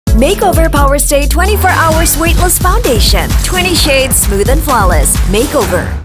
Powerstay 24H Weightless Liquid Foundation - Sunbathe
For you who battle the SUN HEAT almost daily but still want to look 100%…  the NEW Powerstay 24H Weightless Liquid Foundation is your savior 💖✨

⭐️ Zero-smudging staying power
its coverage won’t lose to heat exposure and stay flawless

with extra sun protection to maximize your sunscreen

⭐️ Non oxidizing even at outdoor
the perfect-match shade will stay true no matter the climate